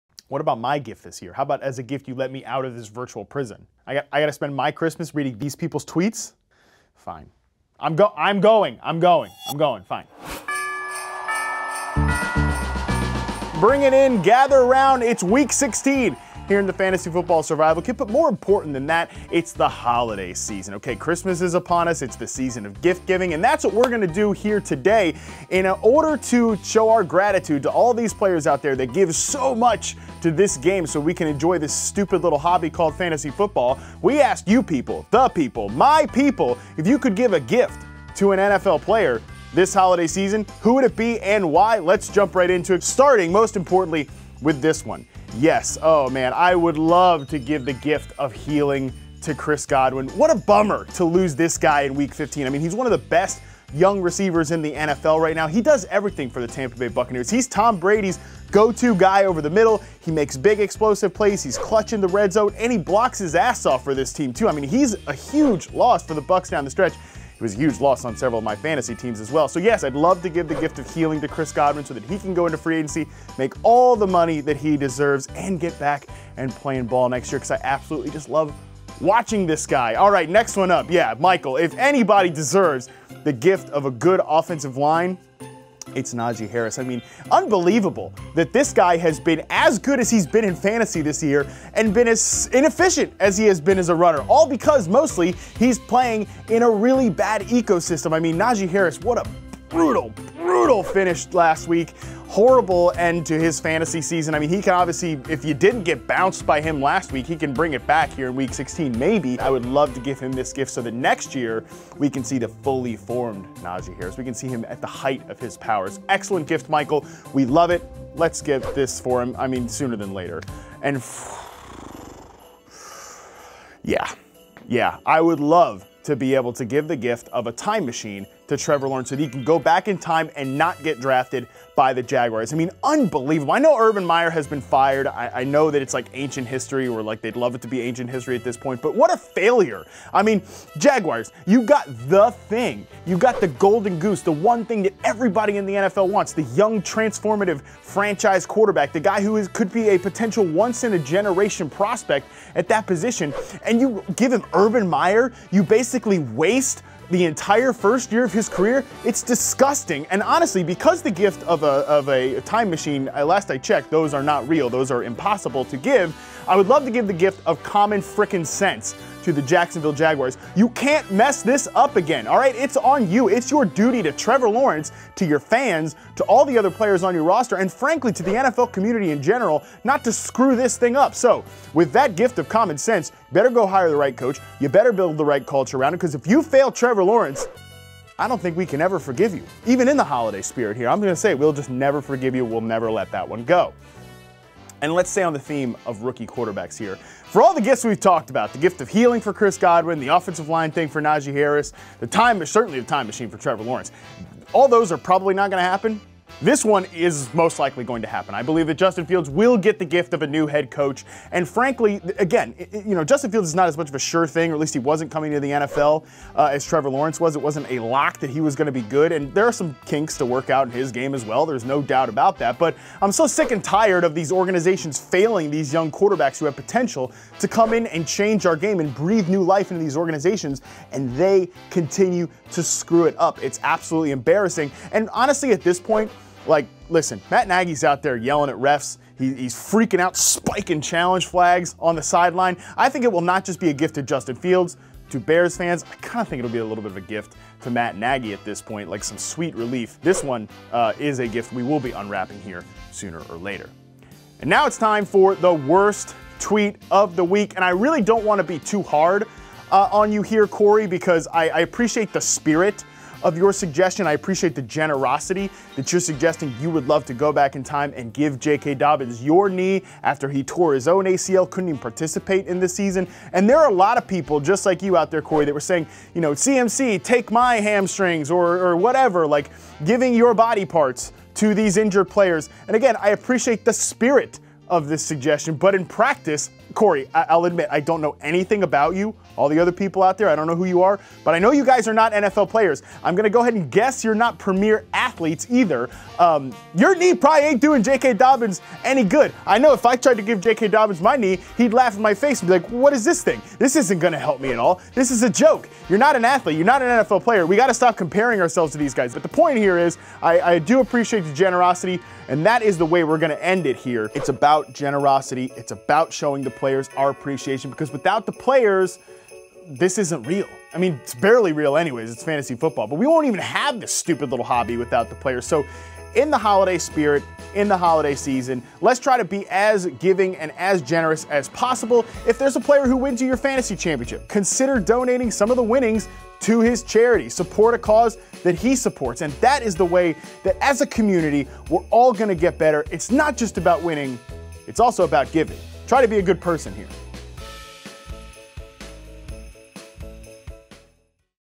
What gift would you give to an NFL player? | FFSK
If you could give one gift to any NFL player, what would it be and why?